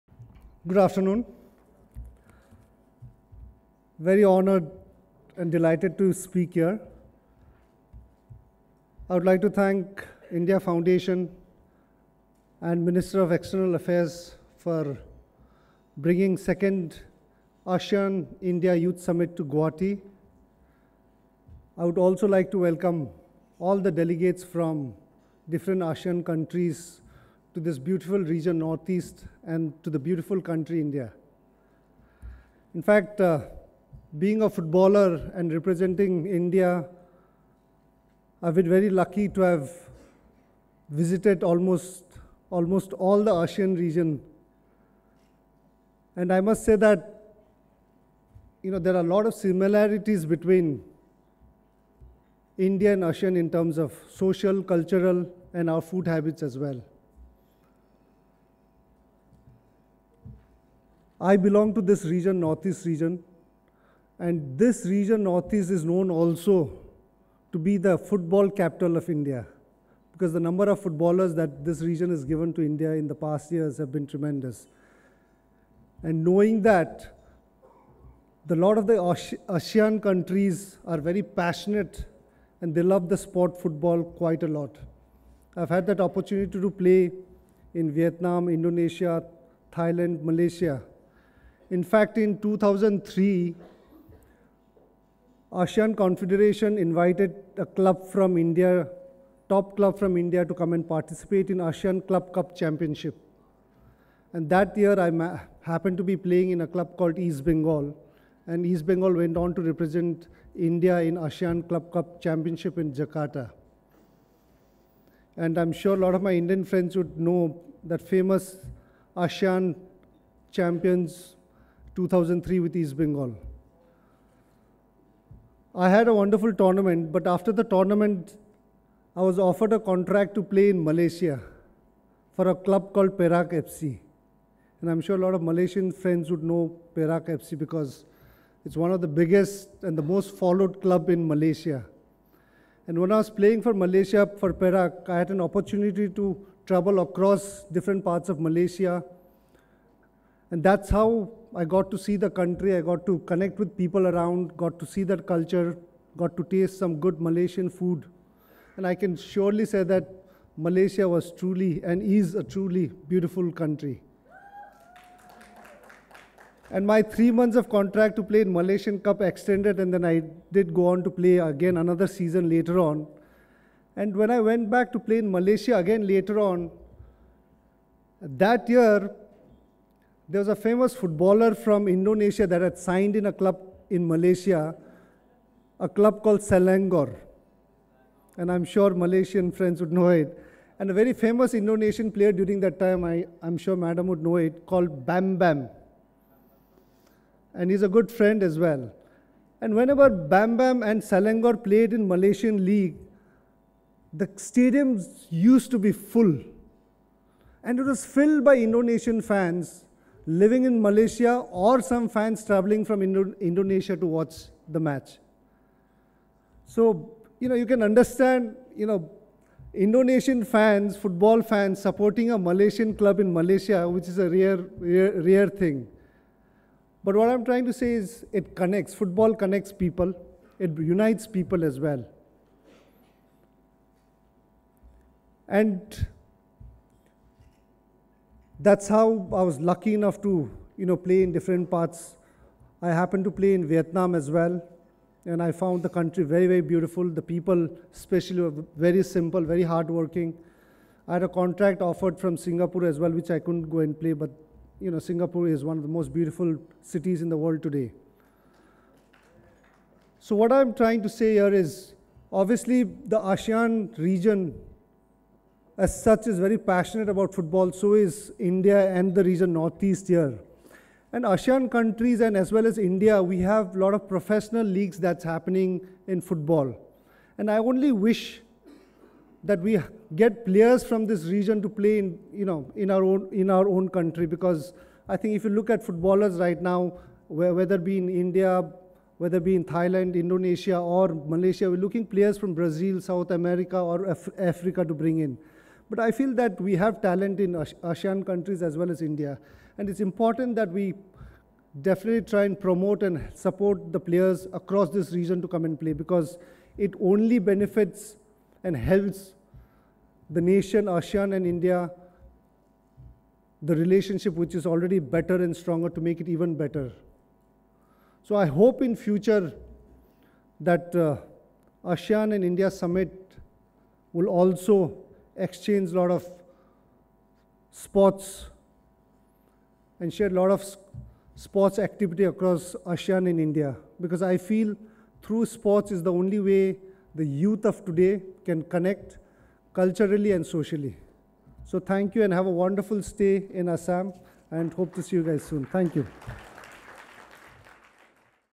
Shri Bhaichung Bhutia at the 2nd ASEAN-India Youth Summit in Guwahati
Shri Bhaichung Bhutia, Footballer & Former Captain of Indian National Team, at the 2nd ASEAN-India Youth Summit in Guwahati